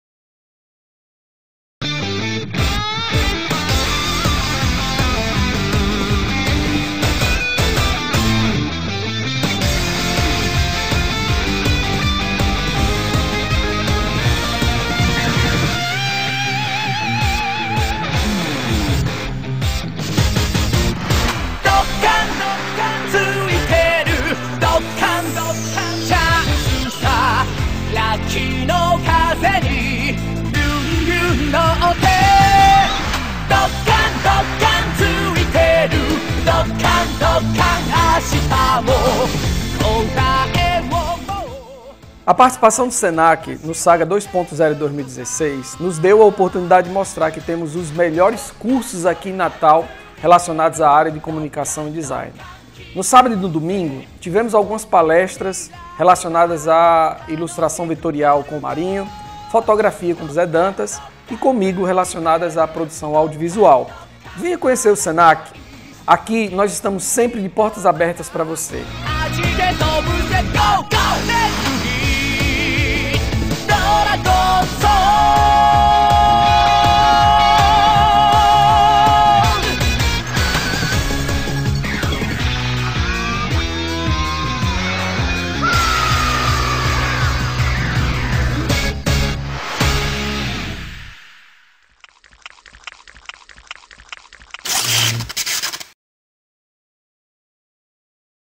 Senac RN no Saga 2.0
Durante os dias 15 e 16 de outubro, o Senac RN participou do Saga 2.0, o maior festival de cultura pop do RN. A instituição ofereceu palestras abordando temas de Design, Fotografia e Vídeo.